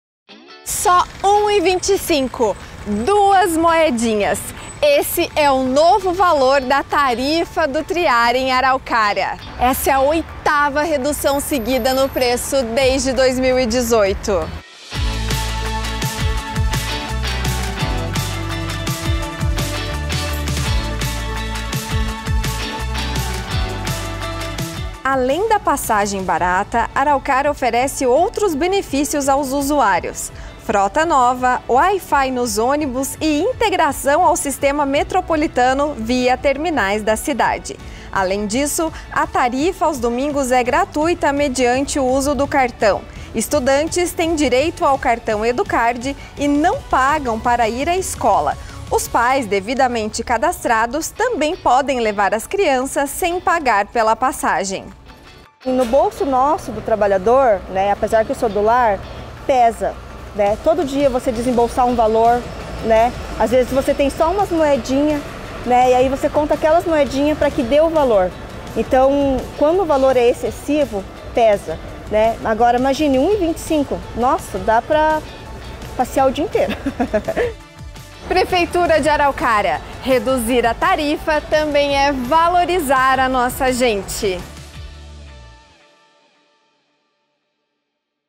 PASSAGEM DO TRIAR BAIXOU DE NOVO
A tarifa do TRIAR custa só R$ 1,25! Duas moedinhas. Essa é a oitava redução desde 2018.